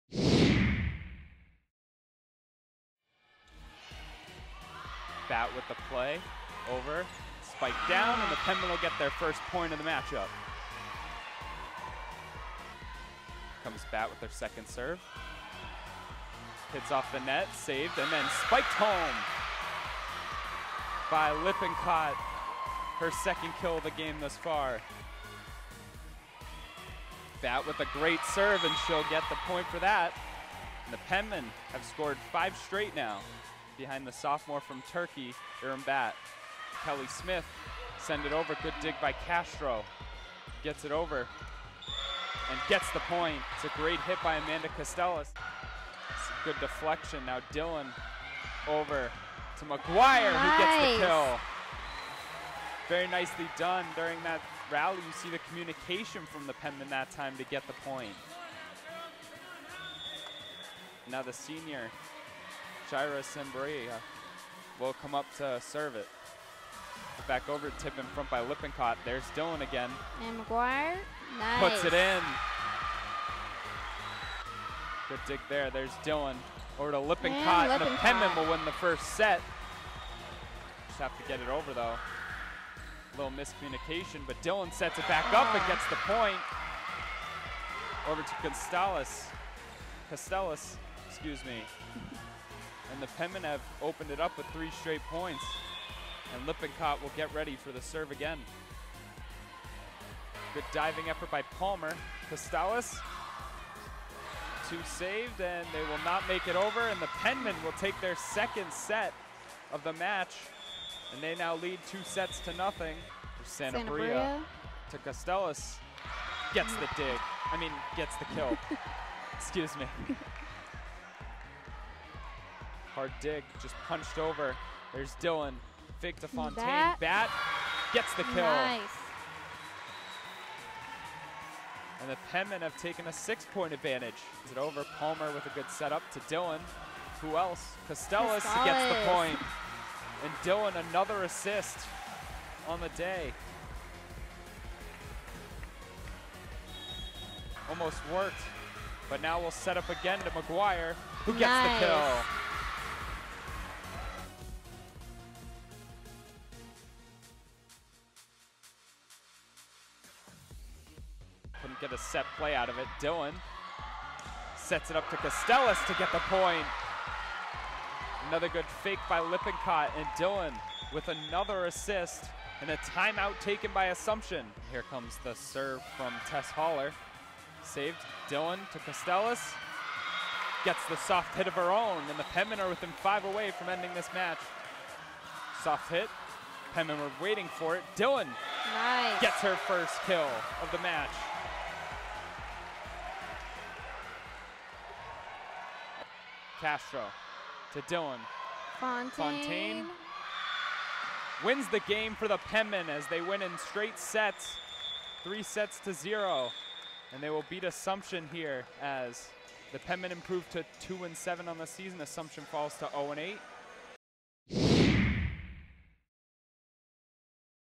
Highlights: @PENMENvball Sweeps Assumption
Baylee Lippincott and Amanda Costales each notched 10 kills to lead the Southern New Hampshire University women's volleyball team to a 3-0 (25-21, 25-18, 25-19) victory in its NE10 home opener Saturday afternoon at the Stan Spirou Field House.